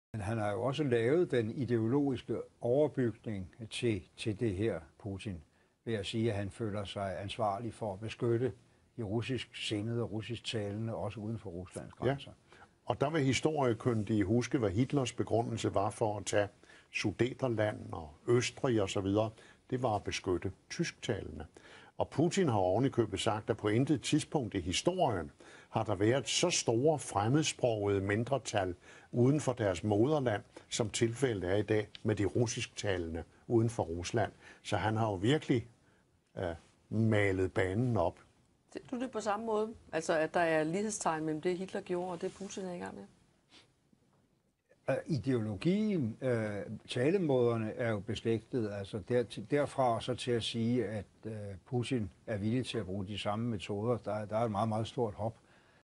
[eng subs] Ellemann & Lykketoft: Putin is Hitler (20150306)
Uffe Ellemann-Jensen, 10 year FM of Denmark in show with Moges Lykketoft, current chairman of the Danish Parliament and next chairman of the UN compares Putin with Hitler.
Ellemann is the predecessor of the leader of the party 'Venstre' and was succeed by Anders Fogh-Rasmussen who became PM of Denmark and later leader of NATO. AFR drove Denmark into the illegal war in Iraq.
UEJ currently have undisclosed interests in business in the Baltics but anybody is free to draw their own conclusions.

TV 2 is state owned media but all media in Denmark 'agrees' that Russia is using propaganda. No critical questions is asked in their weekly one hour show. The show is watched by Danish politicians as it is aired on the Danish equivalent to CNN.
To date Uffe Ellemann-Jensen's opinions are 1:1 with John Kerrys' and the US Department of State.